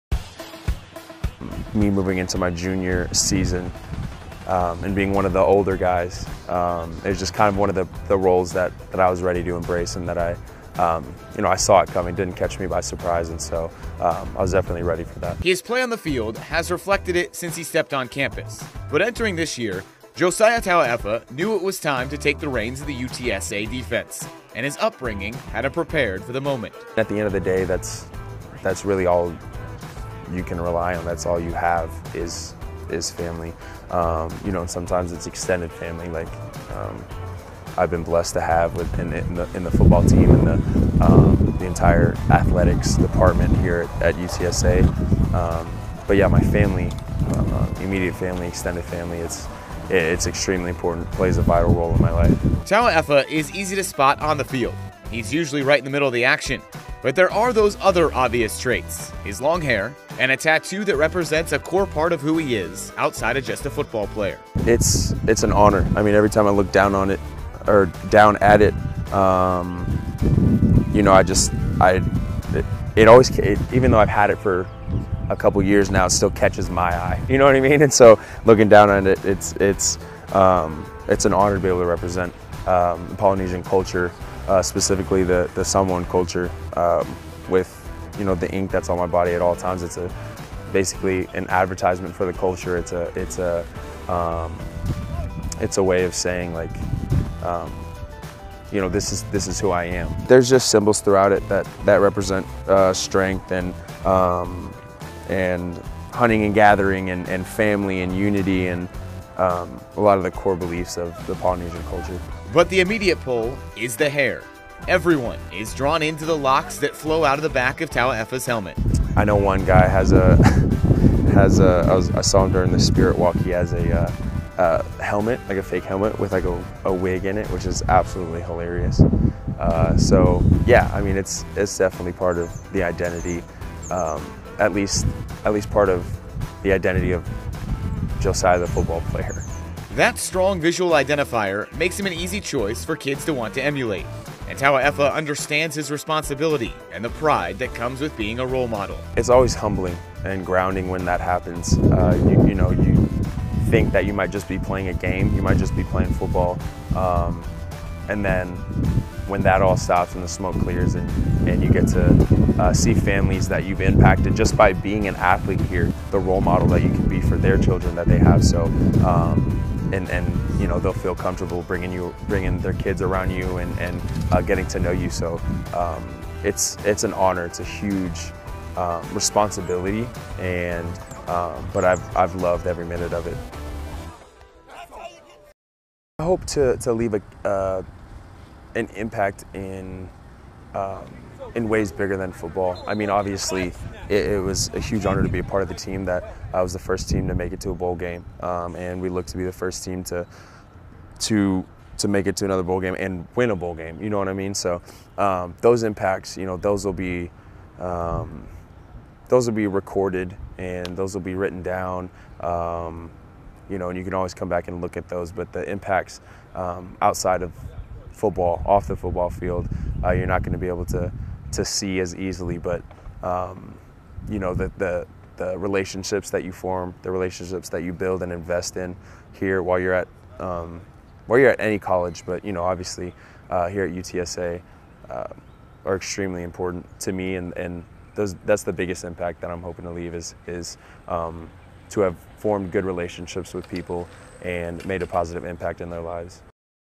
UTSA Football Insider feature: Josiah Tauaefa
One of the more recognizable players on the Roadrunners football team, Josiah Tauaefa is interested in leaving a legacy beyond the gridiron at UTSA.